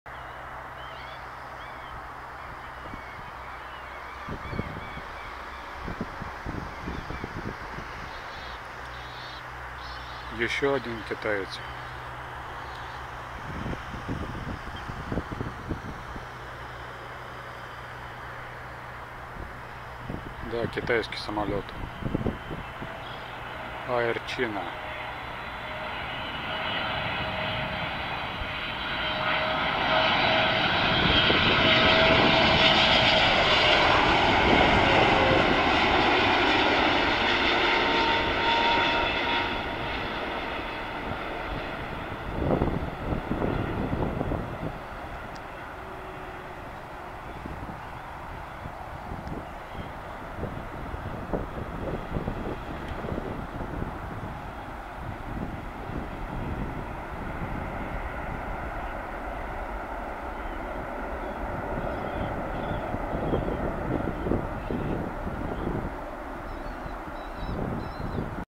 Пекин (Китай) - Москва (Россия). Авиакомпания Air China. Boeing 777-39L(ER). Номер(B-2045)
30.09.23